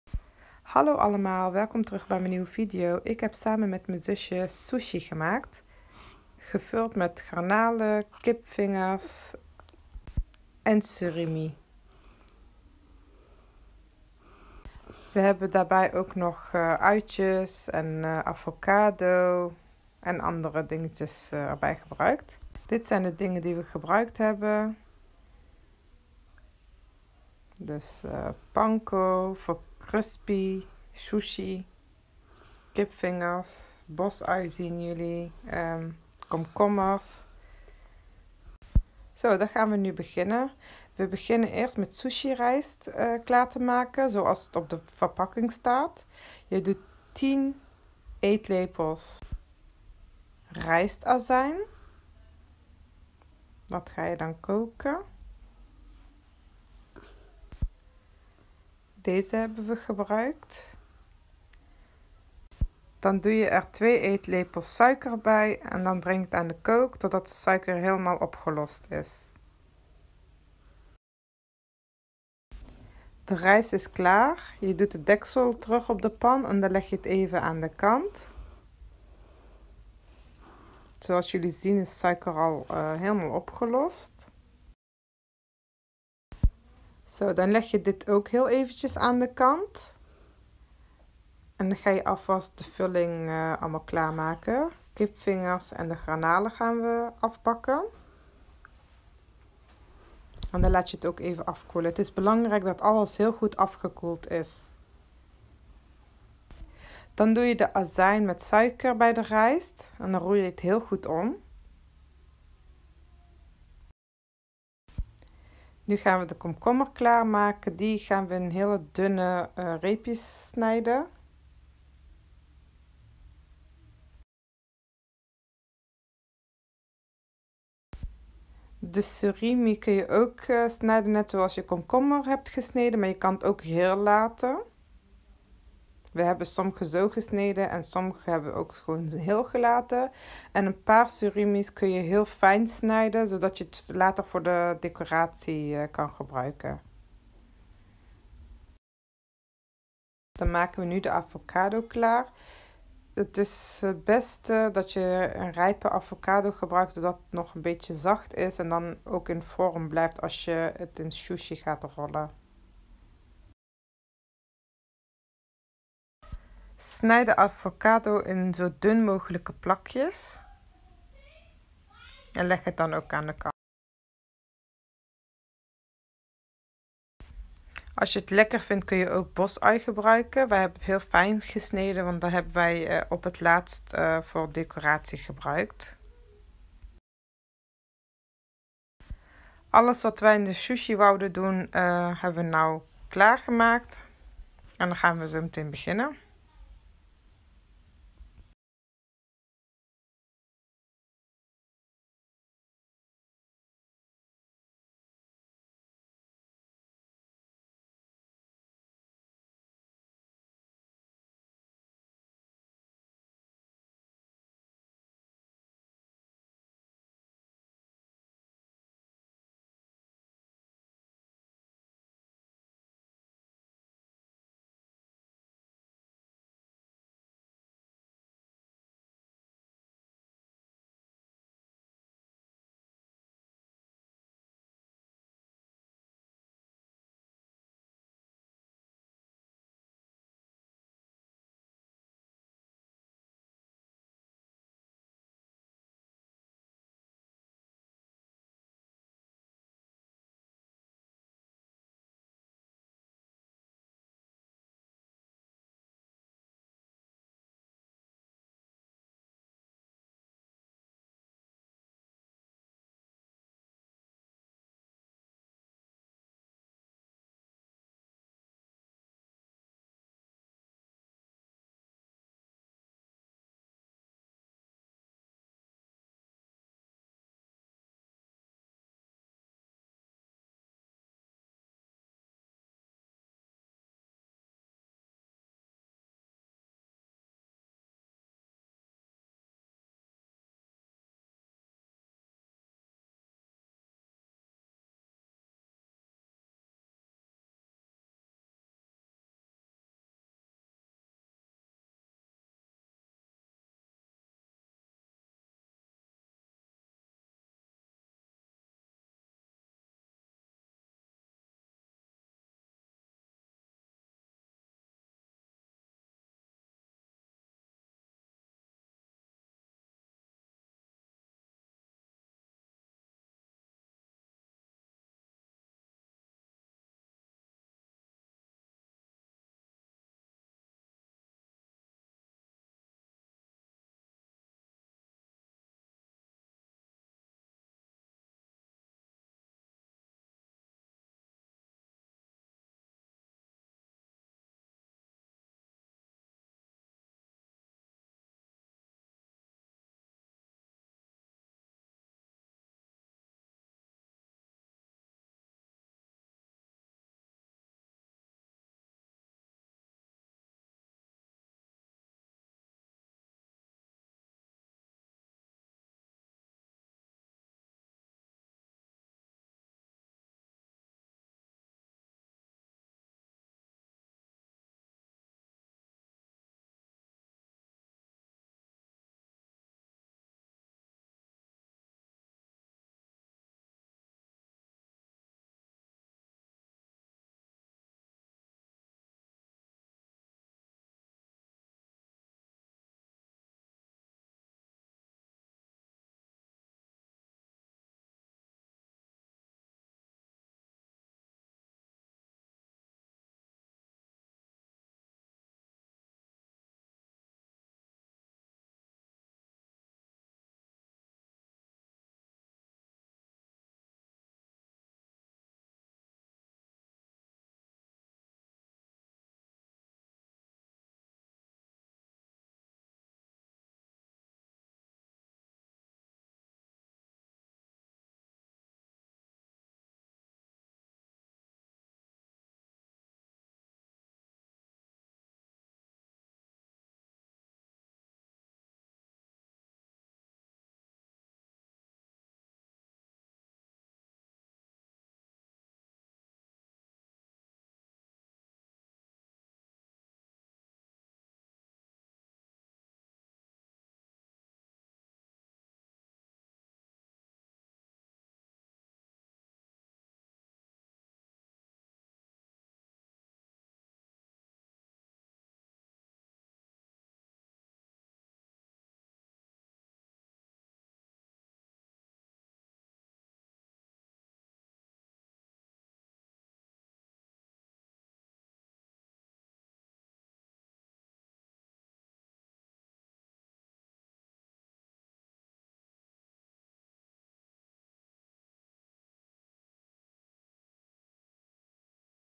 Sushi how to make sushi | Vorbereitung | garnalen | Surimi | kipvingers | recepten oum Younes #75
Je kunt sushi vullen met alles wat je lekker vind. wij hebben de volgende gebruikt in de sushi wat wij gemaakt hebben.
Benodigdheden:
Sushi rijst (rijst koken zoals op de verpakking)
Garnalen
Kipvingers
Surimi
gefrituurde ui
Avocado
Sesam
Zwarte zaad
Panko
Bosui
komkommers
rijst azijn
Suiker
Sushi Nori
Kewpi Mayonaise (super lekkere mayonaise voor bij de sushi)

Bereiding:
In het filmpje kun je zien hoe we het maken

Je kunt mij ook volgen op